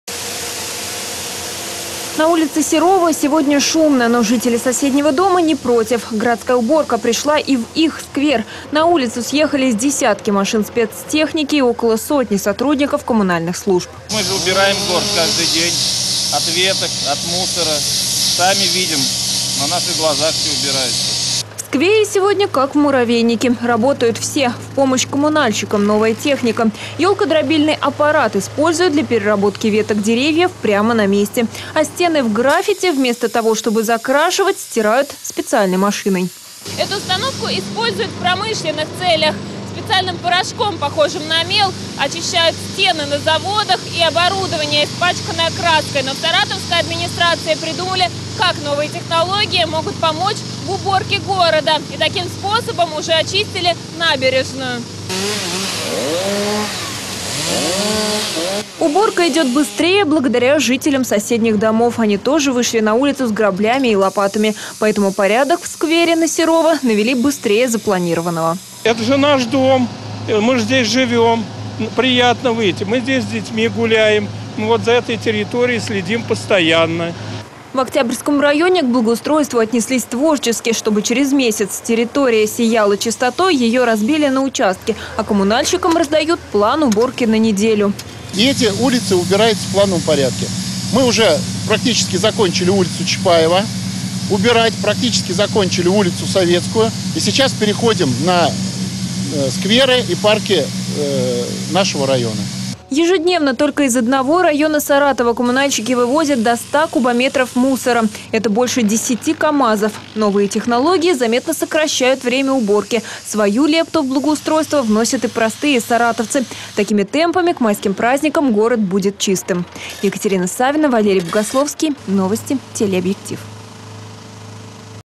В Саратове продолжается благоустройство
Скоро засияет чистотой. В Саратове продолжается благоустройство. Сегодня самые массовые работы были на улице Серова, недалеко от Ильинской площади. Впервые горожане могли наблюдать как со стен смывают графити и убирают ветки с помощью специальных установок. Для наведения порядка начали использовать новые технологии...